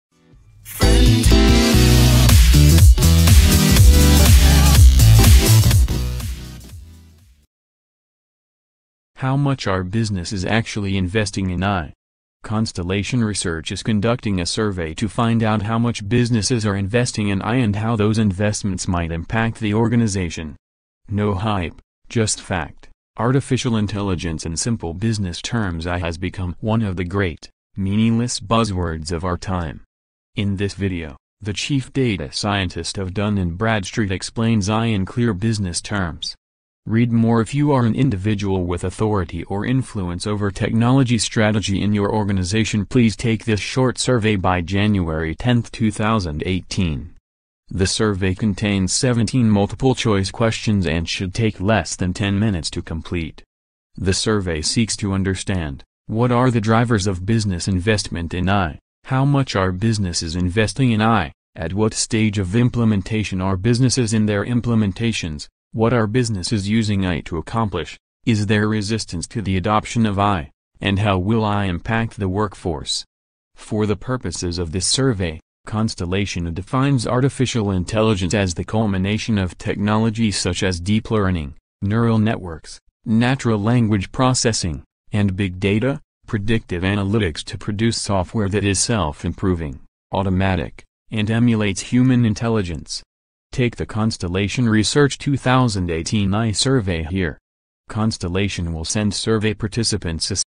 How much are businesses actually investing in AI
Constellation Research is conducting a survey to find out how much businesses are investing in AI and how those investments might impact the organization.

                        No hype, just fact: Artificial intelligence in simple business terms